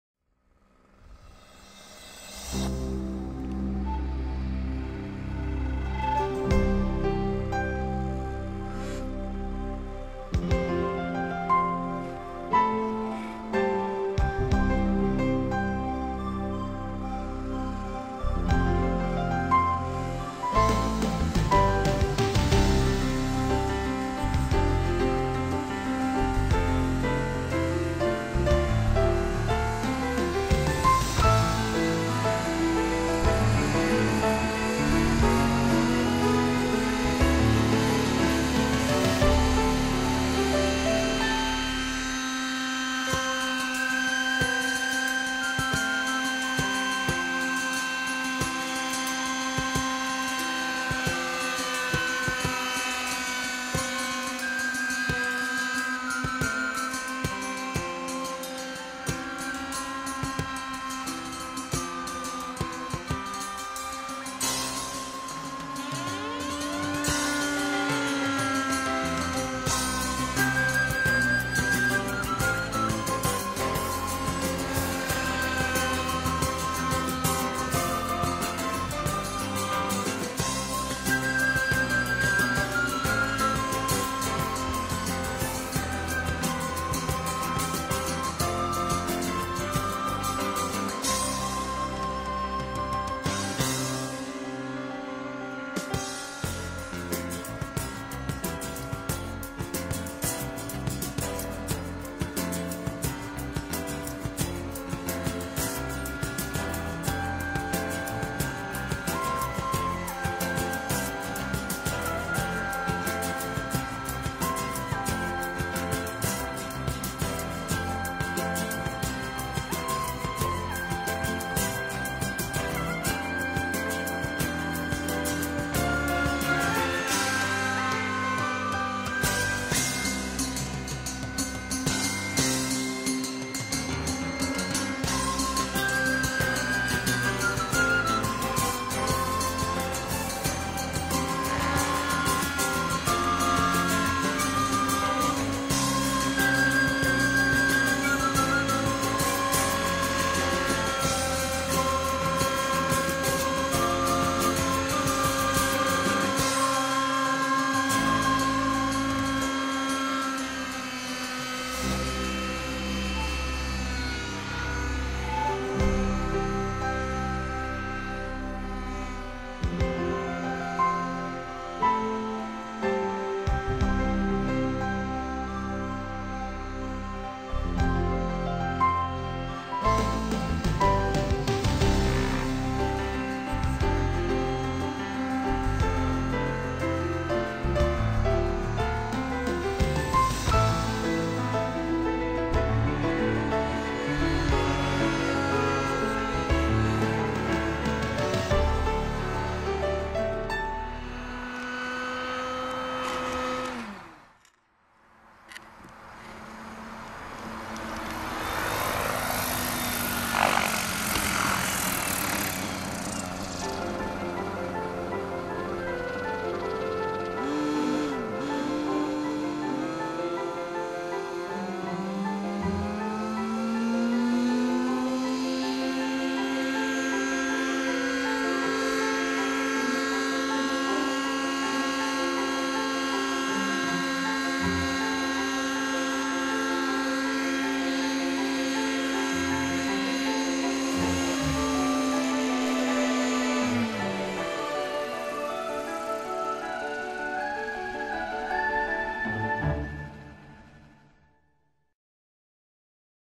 Women Fly Heli's too! Schluter Futura 60
This was a wonderful machine I owned in the late 90's with an O.S. 61H w/pump.  I wish I had it now to convert to electric.  This was about 7 months after my first helicopter flight with a Kalt Baron .60 that I sold.  The unique powertrain in the Futura featured a drive-belt directly from the clutch to a sprocket, which drove the main pinion gear.  This also generated a unique sound that was recognizable from all other helis.